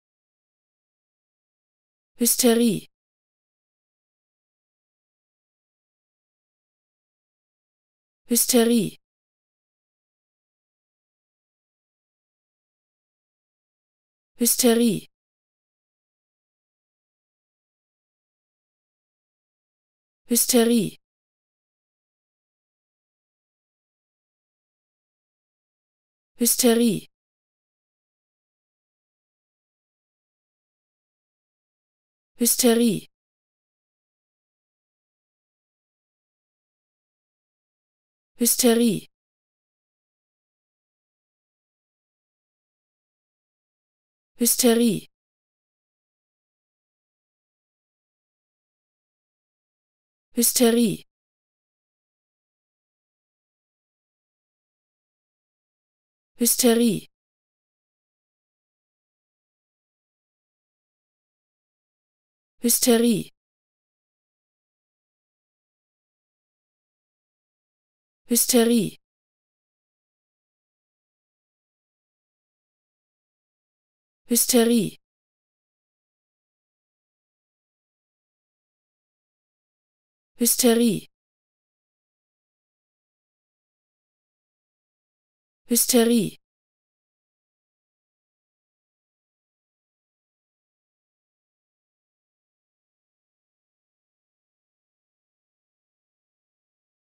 How to Pronounce ''Hysterie'' (Hysteria) Correctly in German
Learn how to say ''Hysterie'' in German with this short pronunciation tutorial.